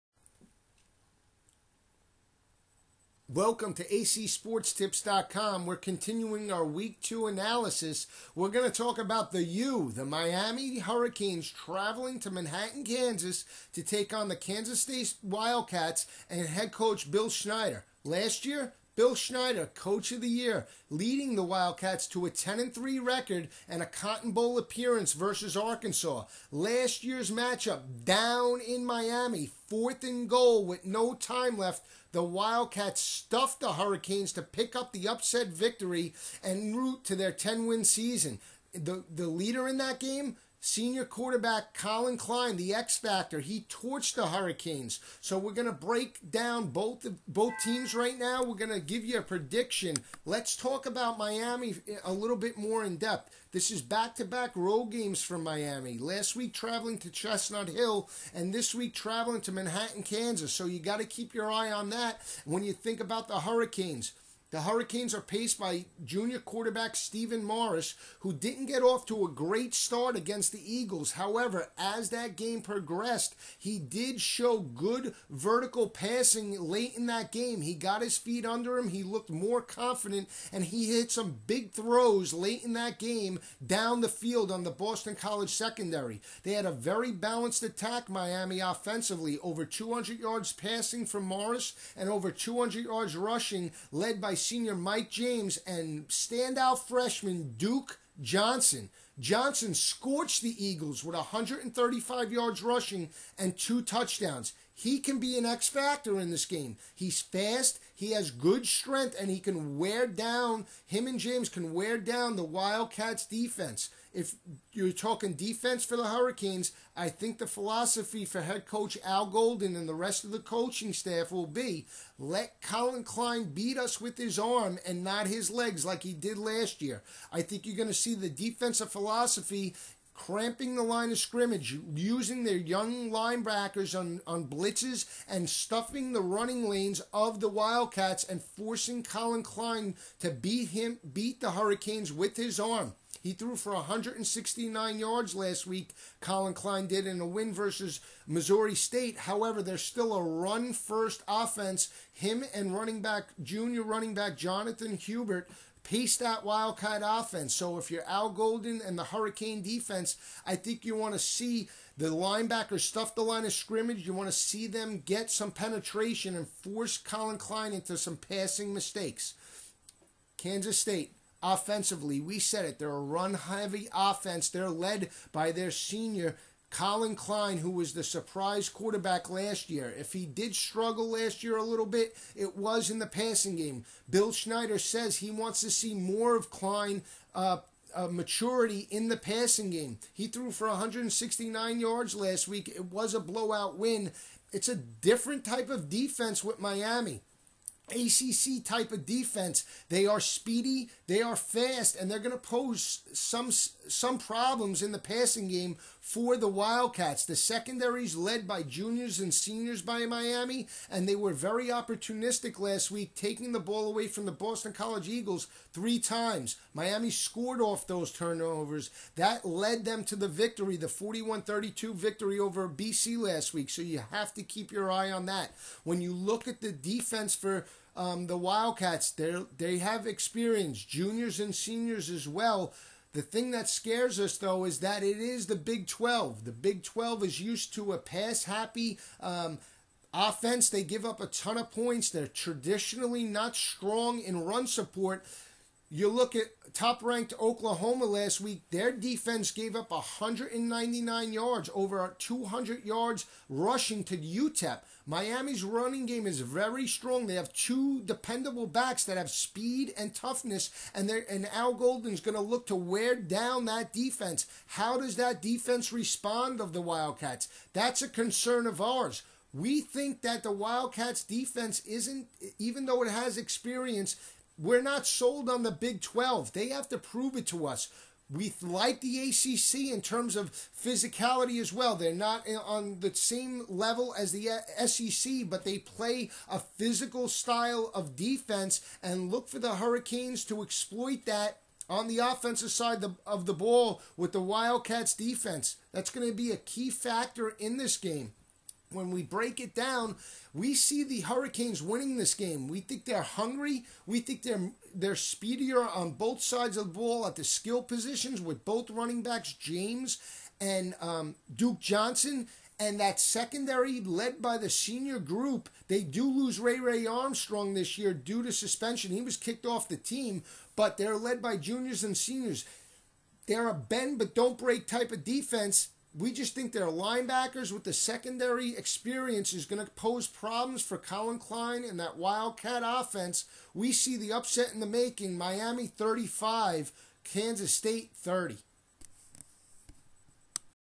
Miami Hurricanes vs Kansas State Wildcats week 2 college football
week 2 college football analysis and prediction of the Miami Hurricans vs Kansas State Wildcats

ncaa week 2 college football predictions